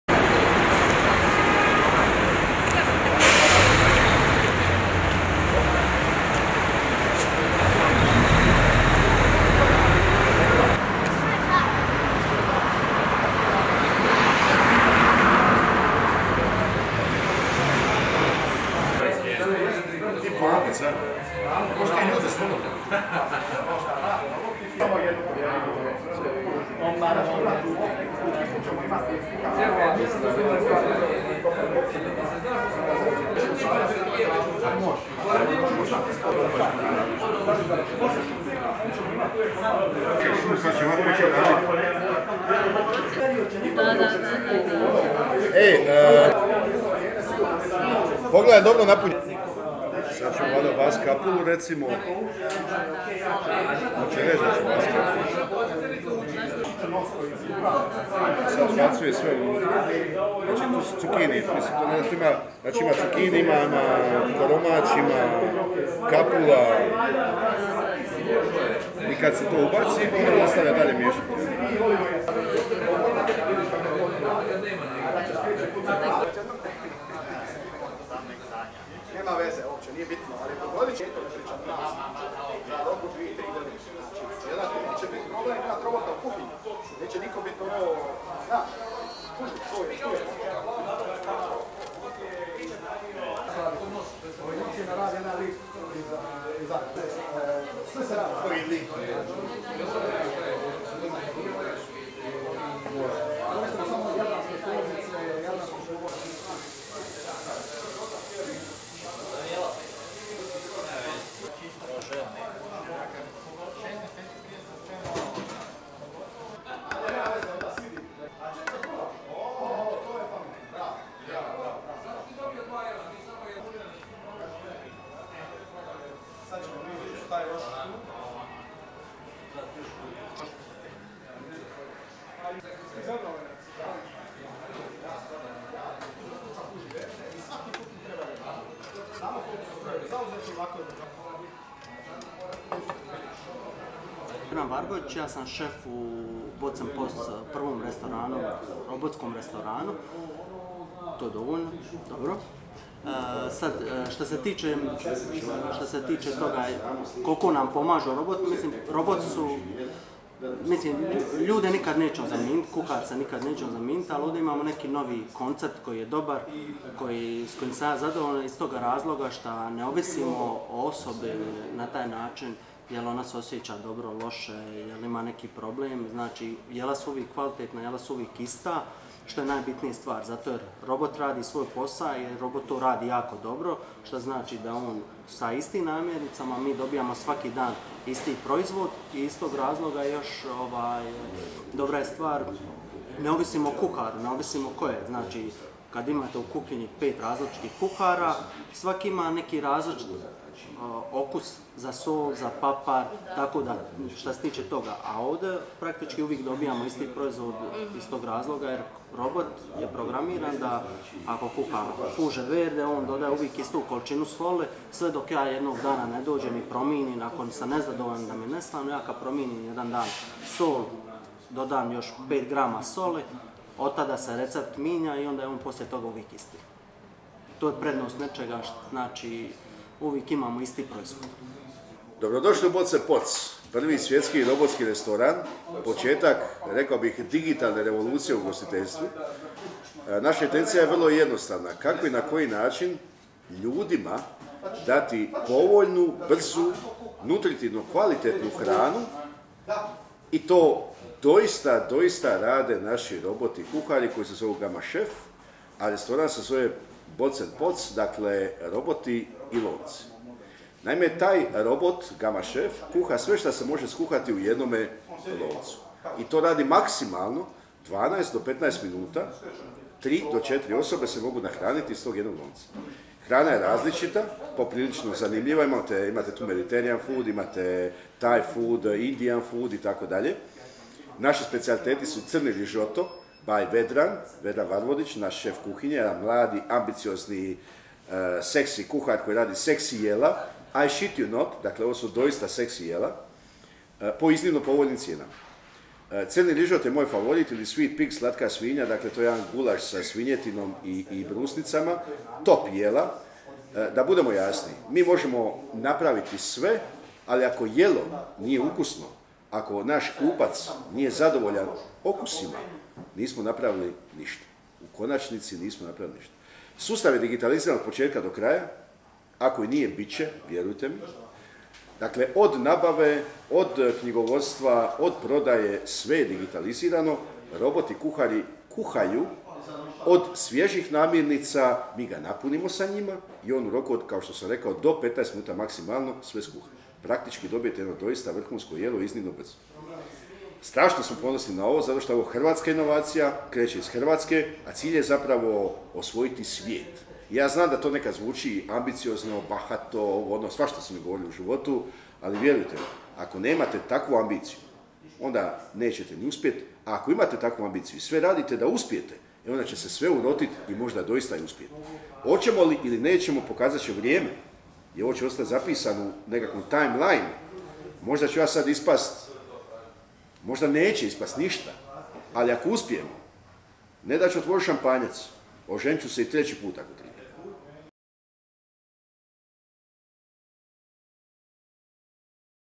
Otvoren prvi robotski restoran u Hrvatskoj Bots&Pots
U Zagrebu je otvoren prvi robotski restoran u Hrvatskoj Bots&Pots. Vlasnik restorana je Hrvoje Bujas.